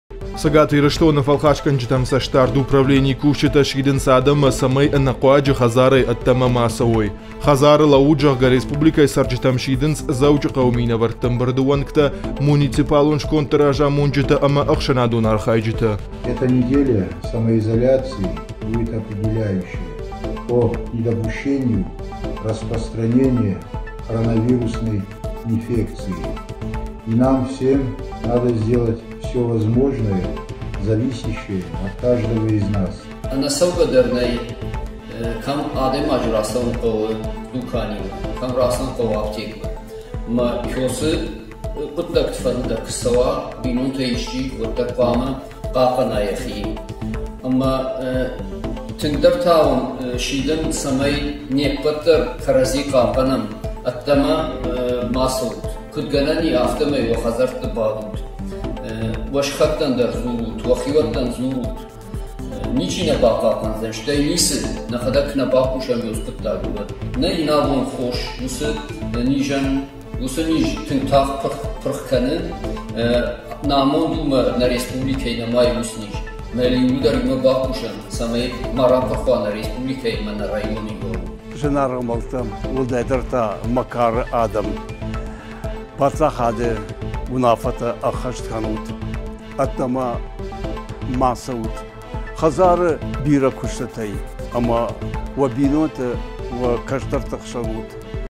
Фæлхасгæнджытæм цæстдарды управленийы кусджытæ сидынц адæммæ, цæмæй хæдзарæй æддæмæ ма цæуой
Цæгат Ирыстоны Фæлхасгæнджытæм цæстдарды управленийы кусджытæ сидынц адæммæ, цæмæй æнæхъуаджы хæдзарæй æддæмæ ма цæуой.

Хæдзары лæуут, зæгъæг, республикæйы цæрджытæм сидынц муниципалон скондты разамонджытæ æмæ æхсæнадон архайджытæ дæр.
Мы рядом в:
ok.ru/irystontv